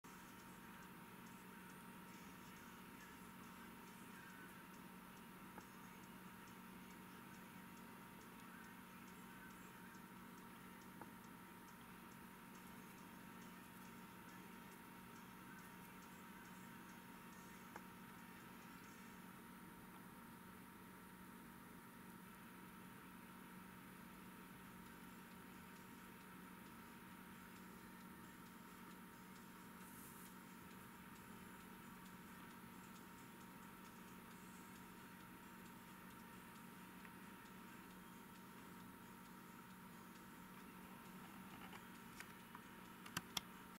Почтовая марка. Почта СССР. 1983 г. Слава Великому Октябрю!
Увеличение и обзор почтовой марки. Почта СССР. 1983 года выпуска. Серия Ленин и Октябрьская Революция. Слава Великому Октябрю! Цена 4 копейки. Прямоугольная. Цветная. Гашеная. Гашение почтовое. Рекомендовано смотреть в Full HD на большом экране.